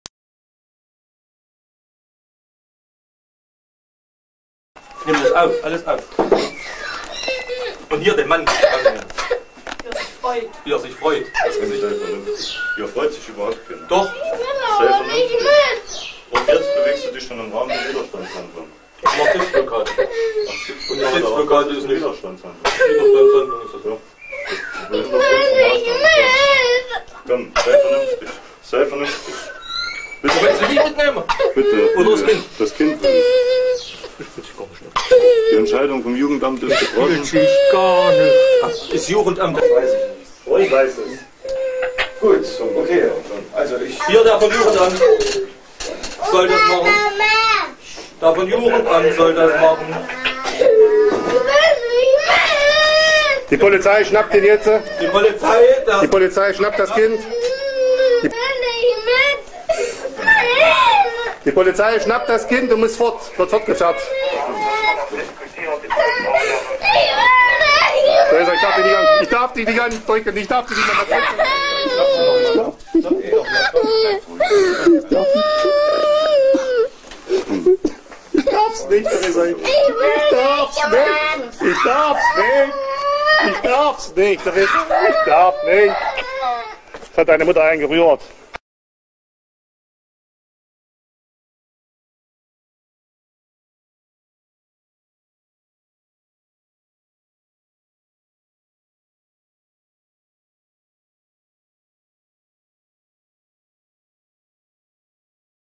Brutaler Kindesentzug durch das Jugendamt Eisenberg
Bei solchen Aktionen muss man an unseren Rechtsstaat zweifeln, hier findet eine akute Kindeswohlgefährdung von Seitens des Jugendamtes des Saale-Holzland-Kreis statt. Das Kind schreit und möchte bei seinem Vater bleiben, auf Befehl der Stasi wird es gewaltsam weggenommen!